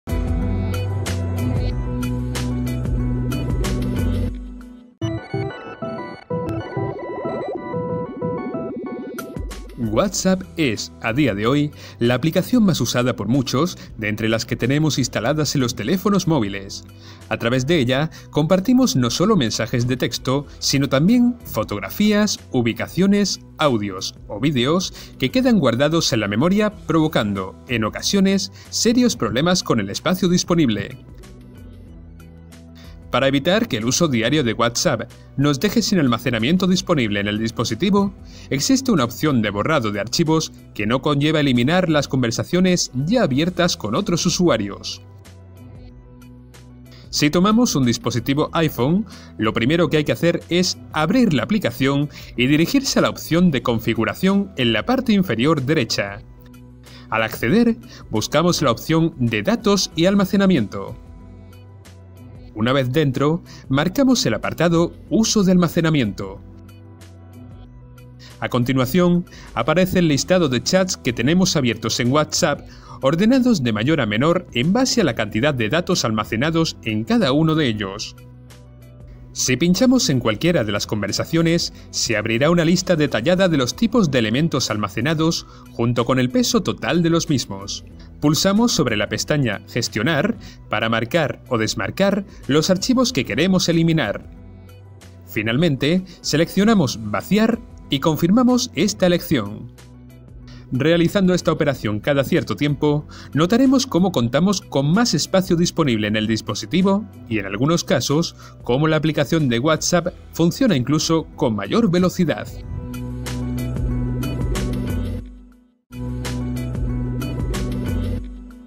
Ponlo En Práctica: Limpiar WhatsApp sin eliminar chats
WhatsApp es, a día de hoy, la aplicación más usada por muchos, de entre las que tenemos instaladas en los teléfonos móviles. A través de ella, compartimos no solo mensajes de texto, sino también fotografías, ubicaciones, audios o vídeos que quedan guardados en la memoria provocando, en ocasiones, serios problemas con el espacio disponible.